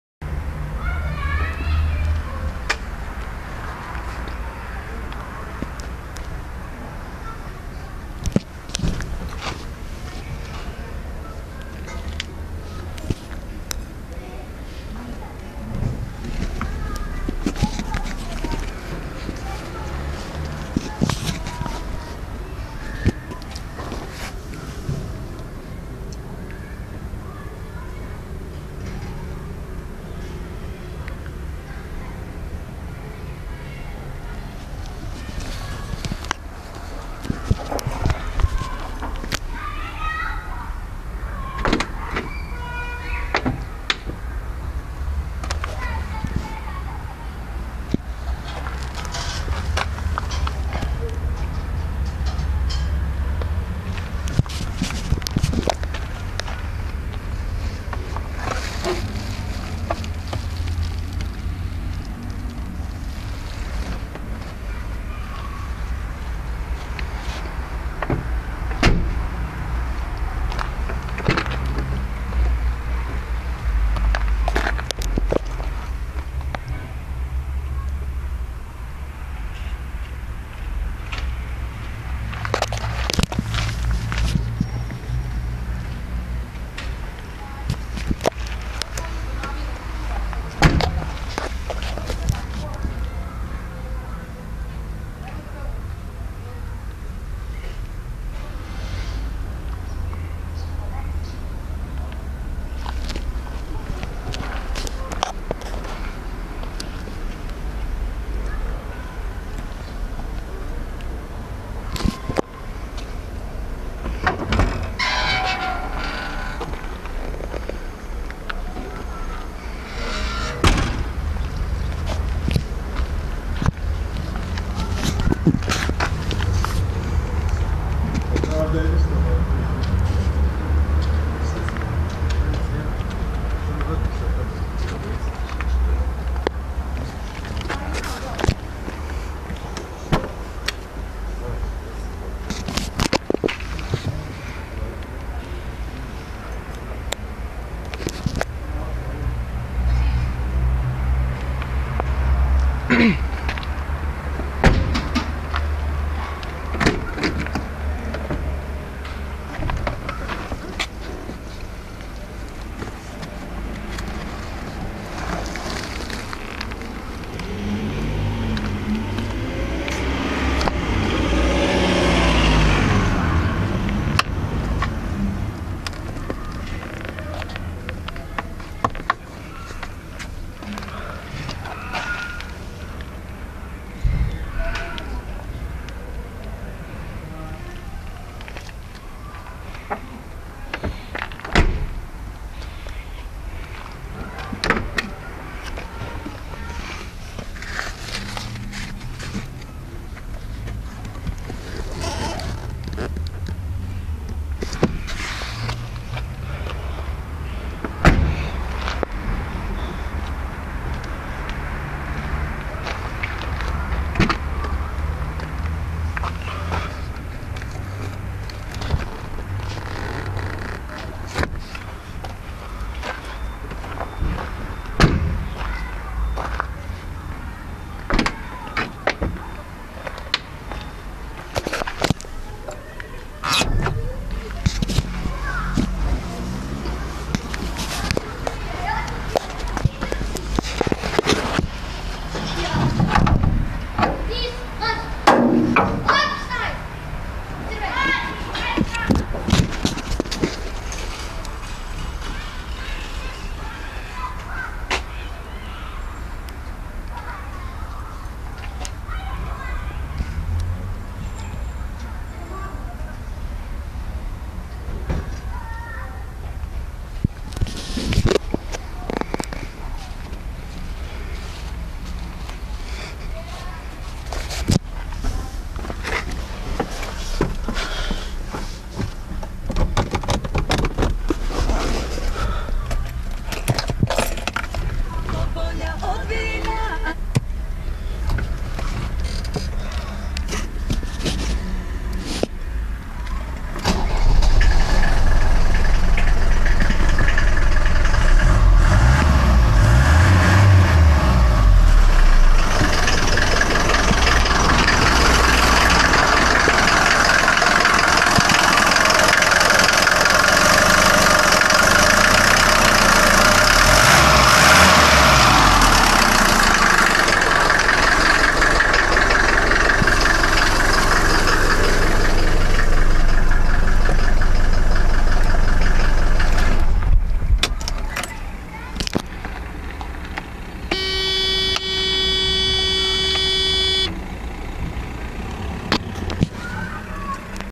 1983 Mercedes-Benz w123 200d Start Up, Engine, & In Depth Tour
1983 Mercedes W123 200D in mint condition. Starts and runs great. Sorry for bad quality.